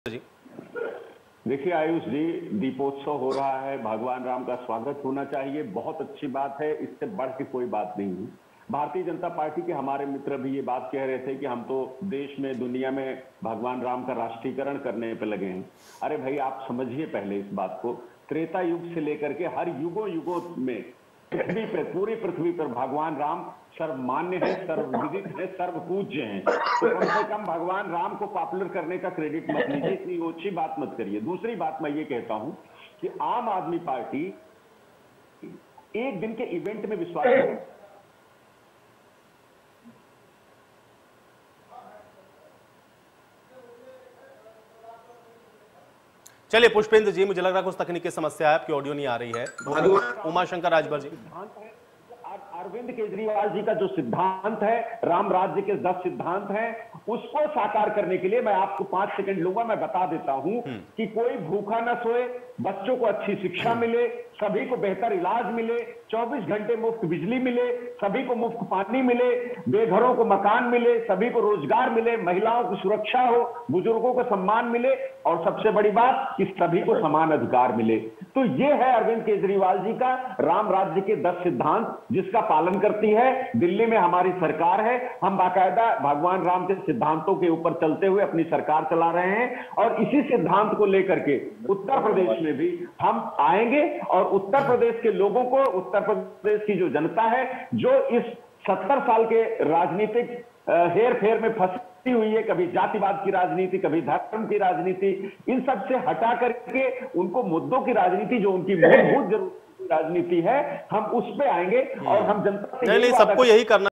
Badi Khabar : आप पार्टी भगवान राम के सिद्धांतों पर चलकर अपनी सरकार चला रही है।
Badi Khabar : आप पार्टी भगवान राम के सिद्धांतों पर चलकर अपनी सरकार चला रही है।  India News UPUK